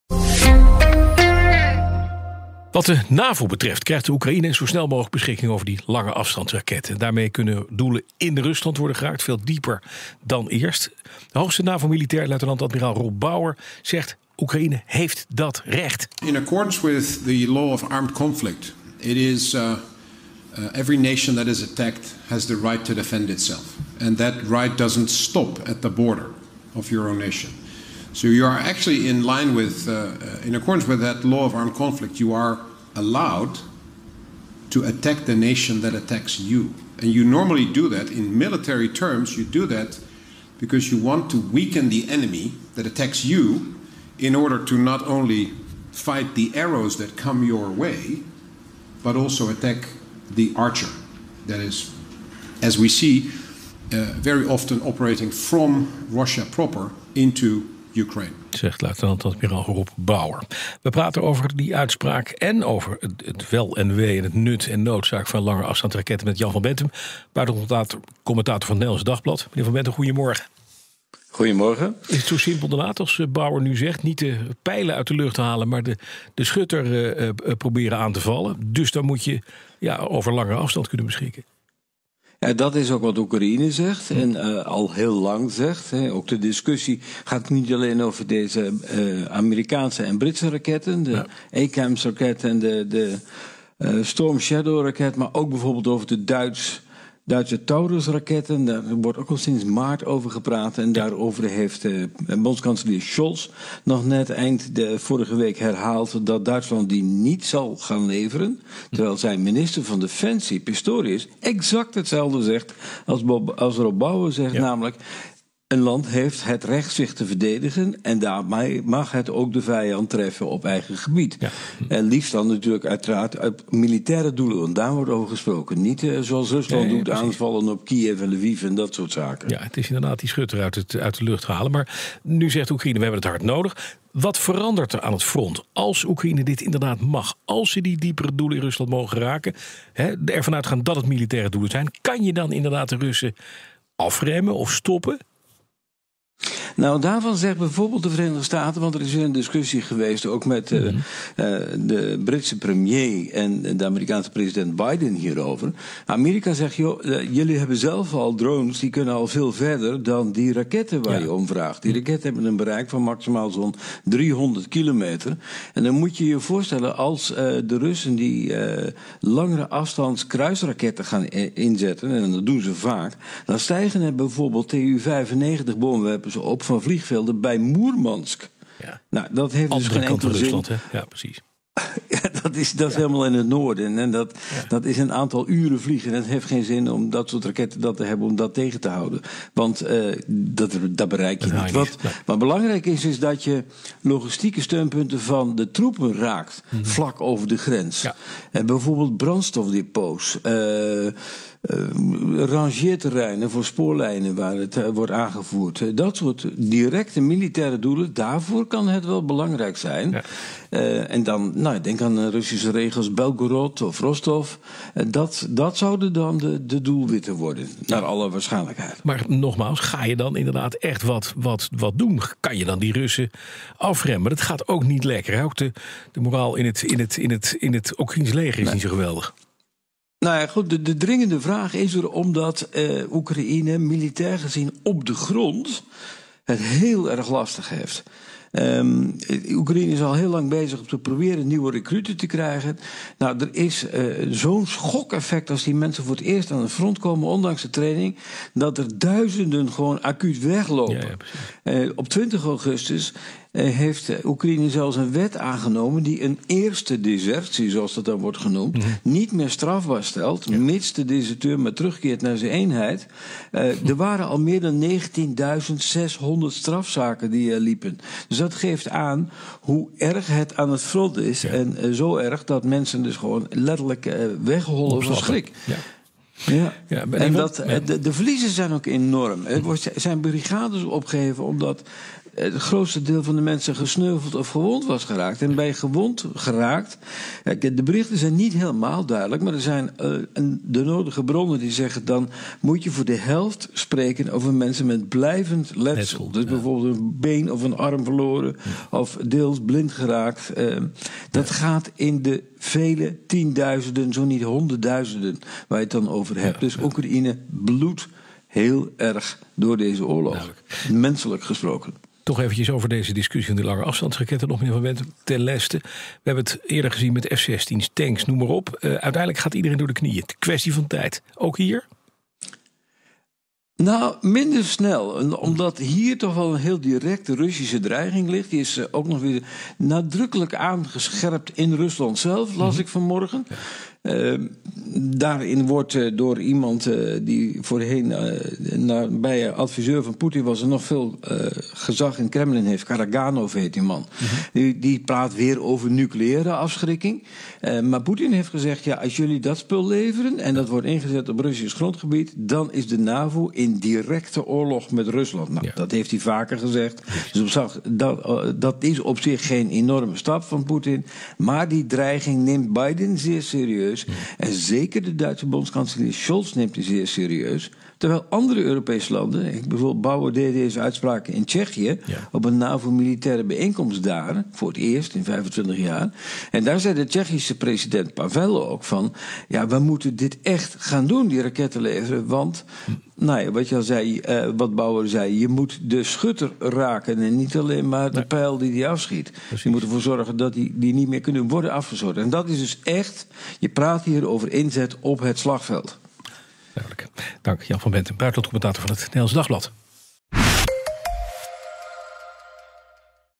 Kyiv wil snel langeafstandsraketten door 'enorme verliezen op de grond'
De roep van Oekraïne om met westerse wapens over de grens in Rusland te mogen aanvallen, is een gevolg van de enorme verliezen die het land lijdt op de grond', stelt Jan van Benthem, buitenlandcommentator van het Nederlands Dagblad. 'Duizenden soldaten lopen gewoon weg van het front.'

Lees het hele artikel op bnr.nl: Kyiv wil snel langeafstandsraketten door 'enorme verliezen op de grond'